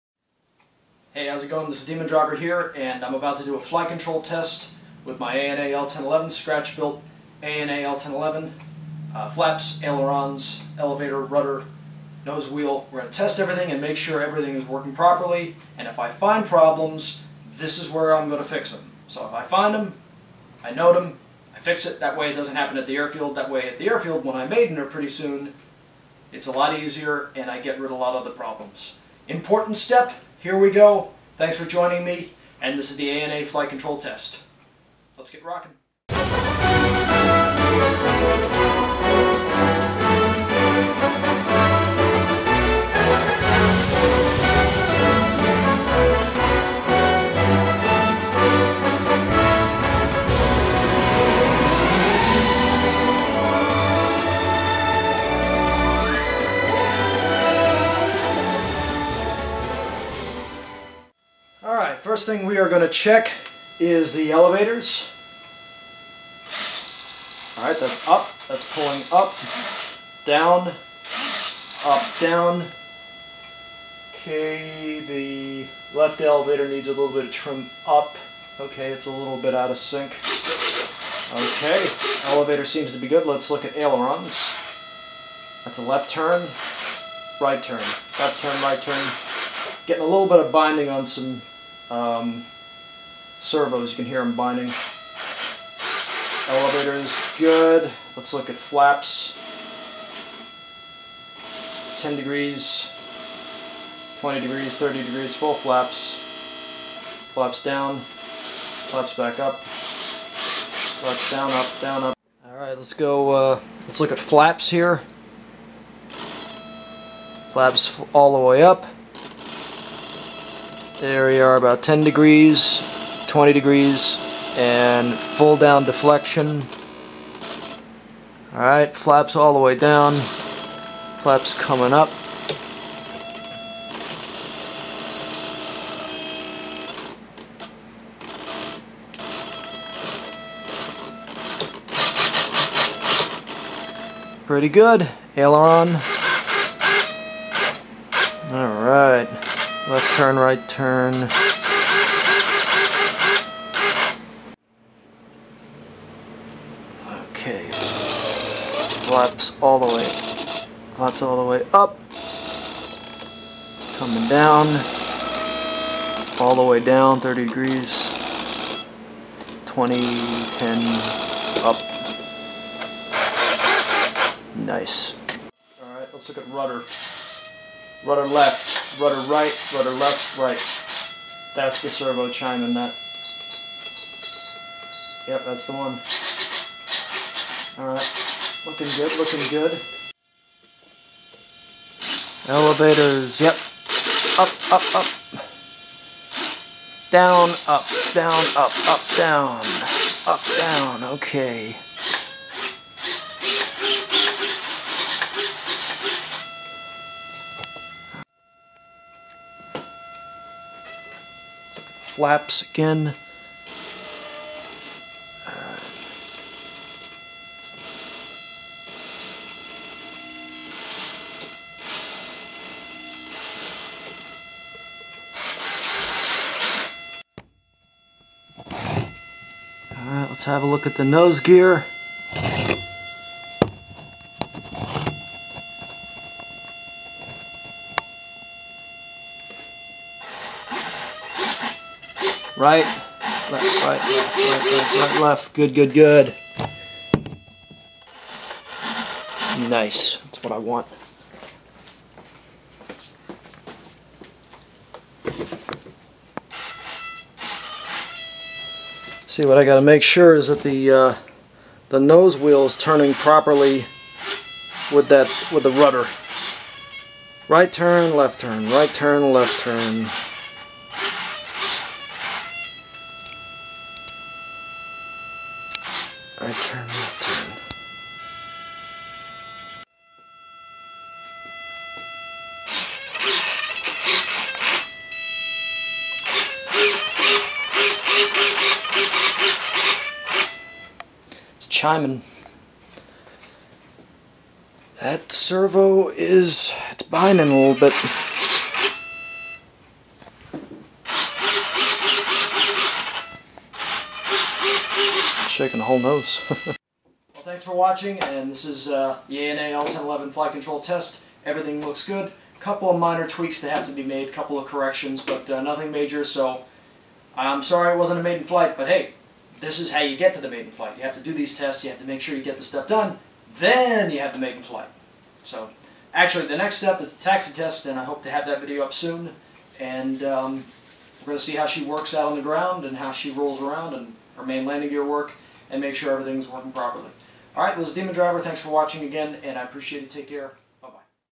RC ANA L-1011 Flight control Test 全日空 L- 1011 フライトコントロールシステム
Hi My friends DemonDriver here and today I wanted to share with you the all important flight control test for my 100% scratch-built RC ANA L-1011 Tristar.  You can't maiden without this test . . . .Period! Enjoy!  フライトコントロールシステム試し!